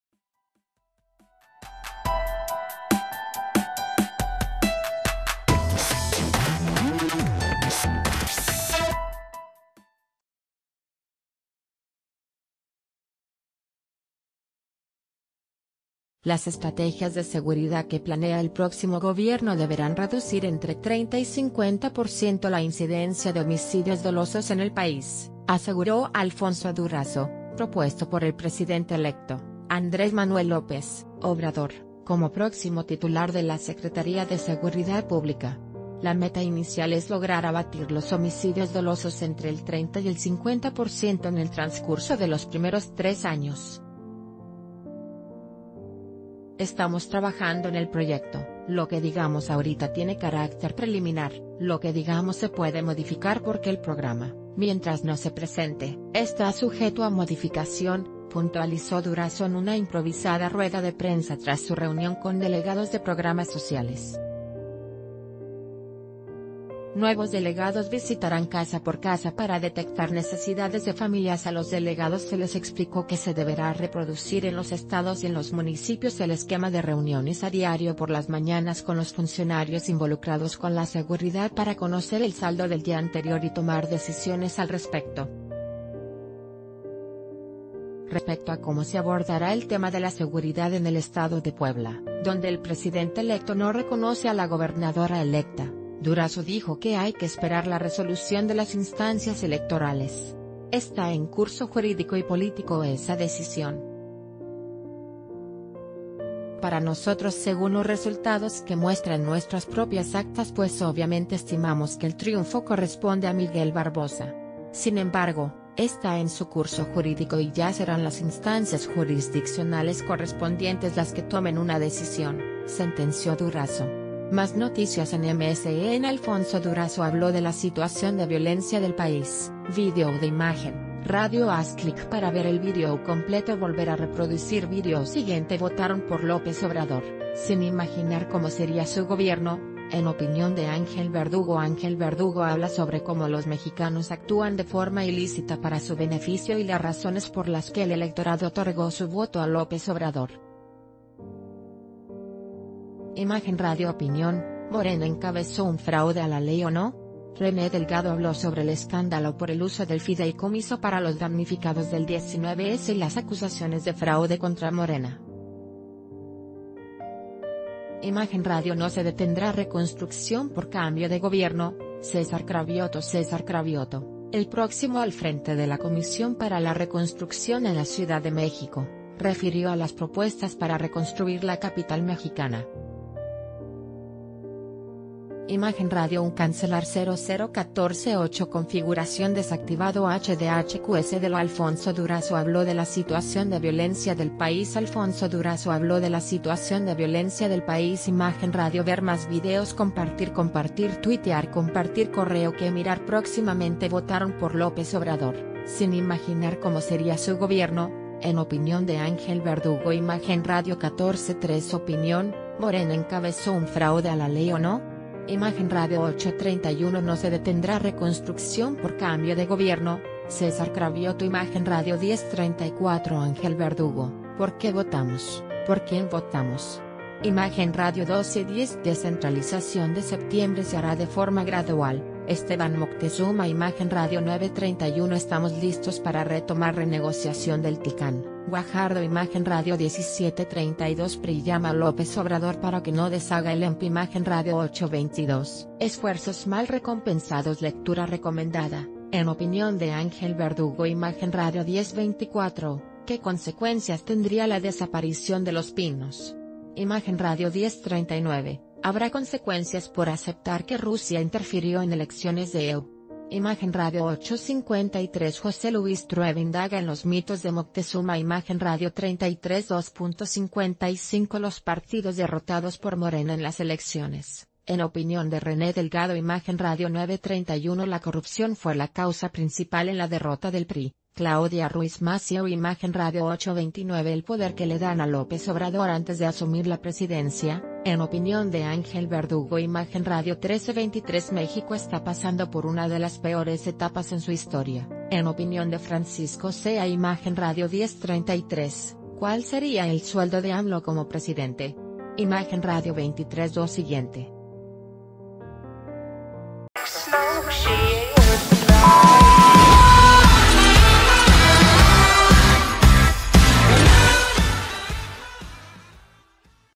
Durazo plantea reducir homicidios entre 30 y 50 por ciento en tres años
Las estrategias de seguridad que planea el próximo gobierno deberán reducir entre 30 y 50 por ciento la incidencia de homicidios dolosos en el país, aseguró Alfonso Durazo, propuesto por el presidente electo, Andrés Manuel López Obrador, como próximo titular de la Secretaría de Seguridad Pública."La...